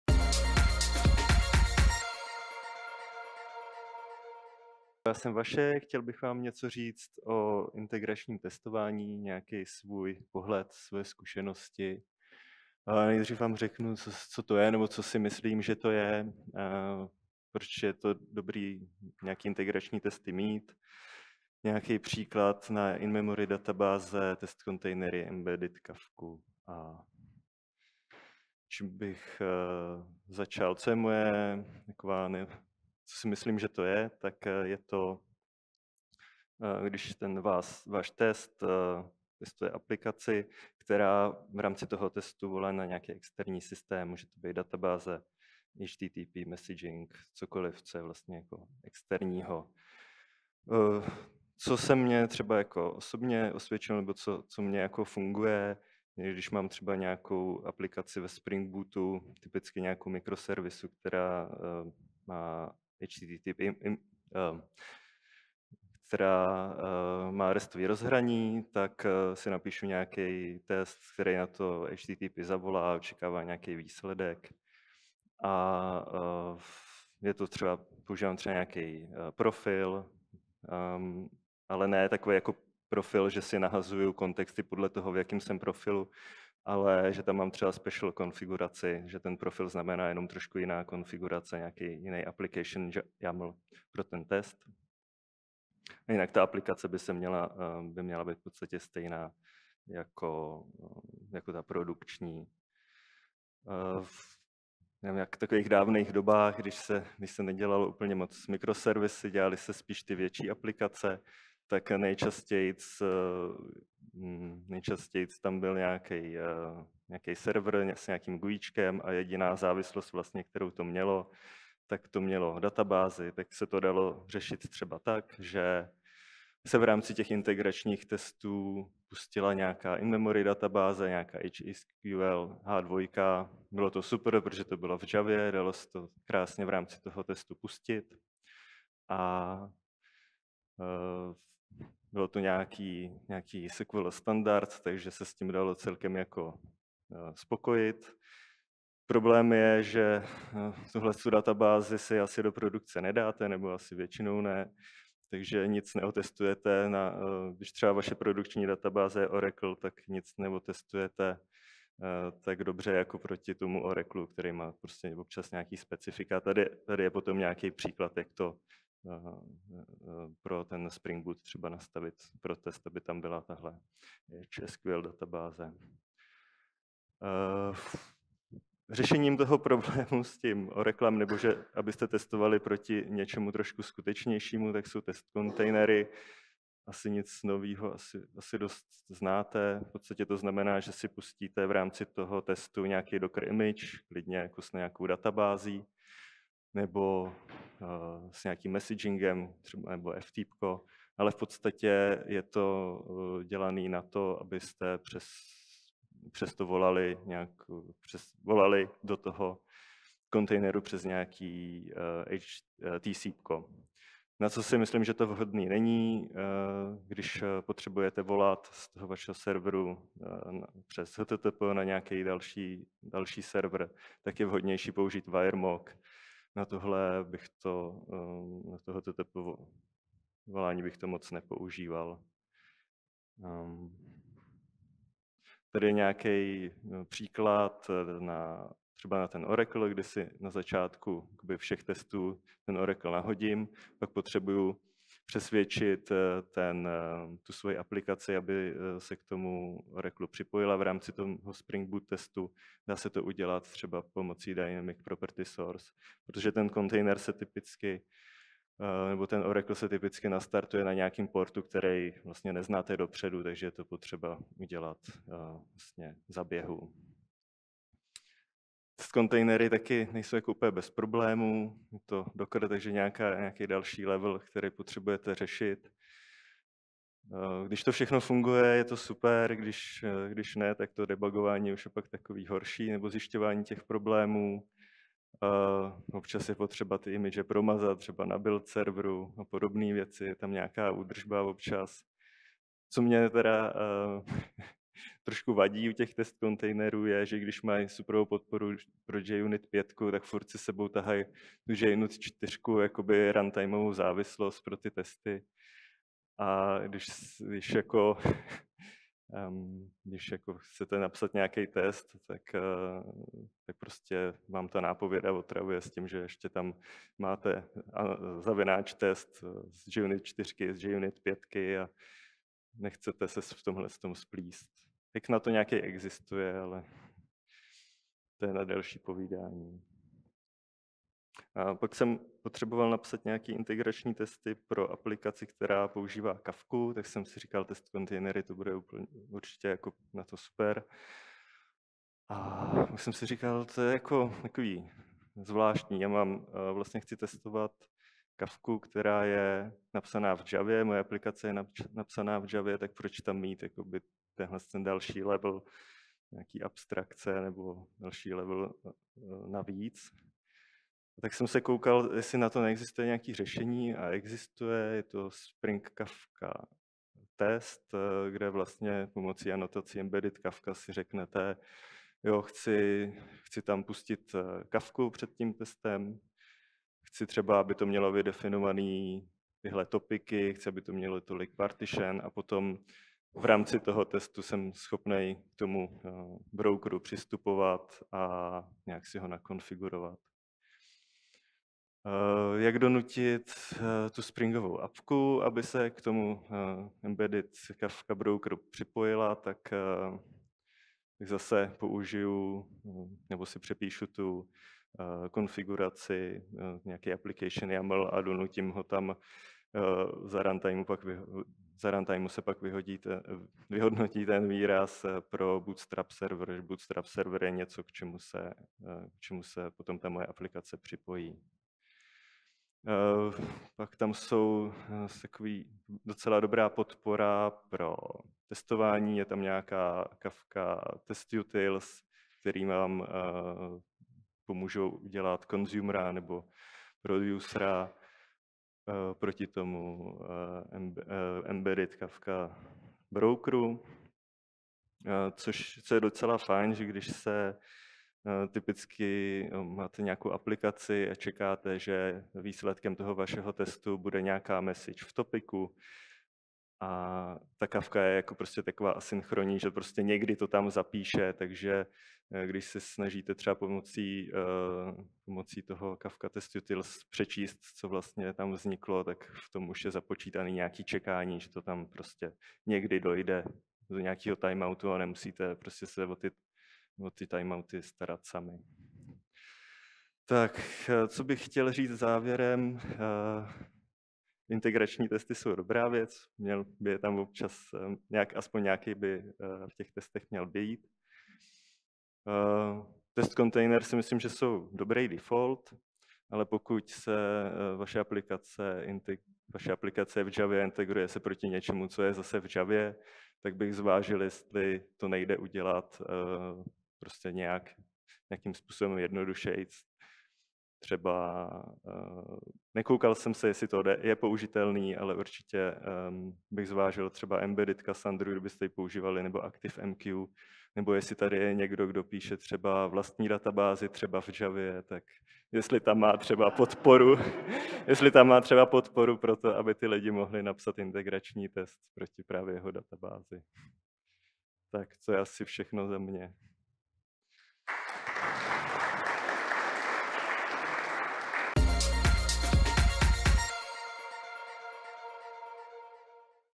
jOpenSpace 2024 - Václav Stolín - Povídání o integračním testování
Přednášející v tomto videu popisuje své zkušenosti a pohled na integrační testování. Na začátku vysvětluje, co integrační testy jsou a proč jsou důležité. Integrační test podle něj testuje aplikaci, která během testování volá externí systém, jako je databáze, HTTP služba nebo messaging systém. Vychází z vlastních zkušeností s aplikacemi postavenými na Spring Bootu, kdy typicky testuje HTTP rozhraní. Při testování se používají různé profily pro mírně odlišné konfigurace, přičemž aplikace by měla být co nejblíže produkčnímu prostředí.

Dále přednášející popisuje historický přístup k testování s in-memory databázemi, jako je H2, a výhody tohoto přístupu, ale také poukazuje na jeho omezení – například že testování s H2 nemusí být přesné pro produkční databáze, jako je Oracle, kvůli jejich specifikům. Jako řešení tohoto problému doporučuje použití nástrojů jako Testcontainers, které umožňují spuštění Docker image databáze během testu.

Testcontainers jsou podle přednášejícího vhodné zejména pro testování databází a messaging systémů, ale zmiňuje i výzvy spojené s jejich používáním, například komplikace s údržbou Docker image na build serverech a složitější debugování.

Dále uvádí příklady práce s Testcontainers v kontextu Spring Bootu, kde pomocí DynamicPropertySource upravuje konfiguraci pro testy s dynamickými porty. Přiznává, že Testcontainers nejsou vždy ideální řešení, zejména pokud je testovaný systém napsán v Javě, a radí zvážit jednodušší alternativy, jako je použití embedded databází či messaging systémů.

Na závěr zmiňuje vlastní zkušenosti s integračním testováním aplikace s použitím Kafky. Objevil, že místo Testcontainers je pro tento účel vhodnější použít Spring Kafka test s anotací EmbeddedKafka, která umožňuje testování bez nutnosti spouštění Kafky v Dockeru. Toto řešení je podle něj jednodušší a efektivnější, přičemž Spring Kafka test poskytuje i užitečné utility pro práci s daty v Kafce během testování.

V závěru přednášející zdůrazňuje, že integrační testy jsou důležitou součástí vývoje, doporučuje je pravidelně používat a zvážit různé přístupy podle toho, jaké technologie aplikace používá.